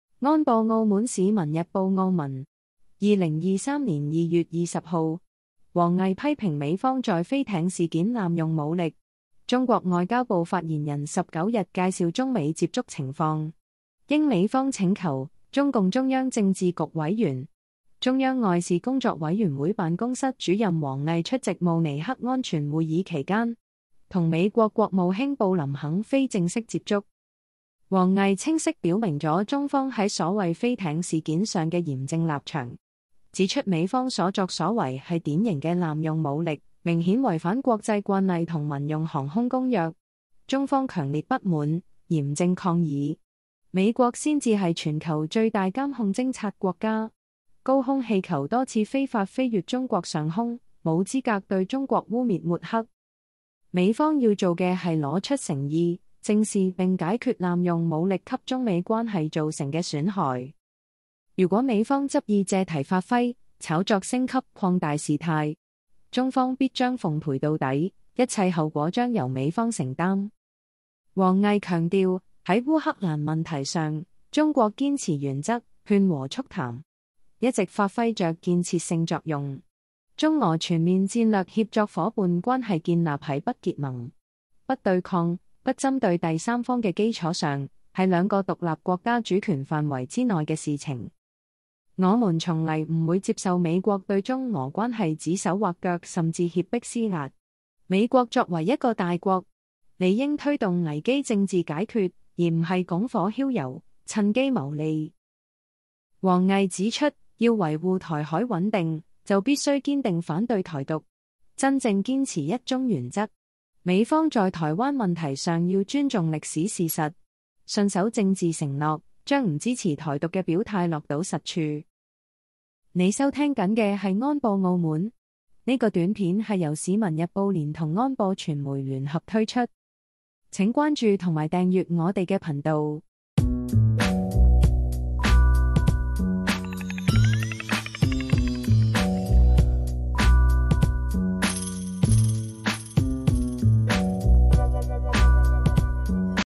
【安播澳門】 王毅批評美方在飛艇事件濫用武力 | 市民日報 –澳門新聞 20230220
王毅 批評美方在飛艇事件濫用武力
中國外交部 發言人19日介紹中美接觸情況。應美方請求，中共中央政治局委員 、中央外事工作委員會 辦公室主任王毅出席 慕尼黑安全會議期間，同 美國國務卿 布林肯 非正式接觸。

　　王毅清晰表明了中方在所謂飛艇事件上的嚴正立場，指出美方所作所為是典型的濫用武力，明顯違反國際慣例和民用航空公約，中方強烈不滿，嚴正抗議。美國才是全球最大監控偵察國家，高空氣球多次非法飛越中國上空，沒有資格對中國污衊抹黑。美方要做的是拿出誠意，正視並解決濫用武力給中美關係造成的損害。如果美方執意借題發揮、炒作升級、擴大事態，中方必將奉陪到底，一切後果將由美方承擔。

　　王毅強調，在烏克蘭問題上，中國堅持原則、勸和促談，一直發揮著建設性作用。中俄全面戰略協作夥伴關係建立在不結盟、不對抗、不針對第三方的基礎上，是兩個獨立國家主權範圍之內的事情。我們從不接受美國對中俄關係指手畫腳甚至脅迫施壓。美國作為一個大國，理應推動危機政治解決，而不是拱火澆油，趁機牟利。

　　王毅指出：要維護台海穩定，就必須堅定反對「台獨」，真正堅持一中原則。美方在台灣問題上要尊重歷史事實，信守政治承諾，將不支持「台獨」的表態落到實處。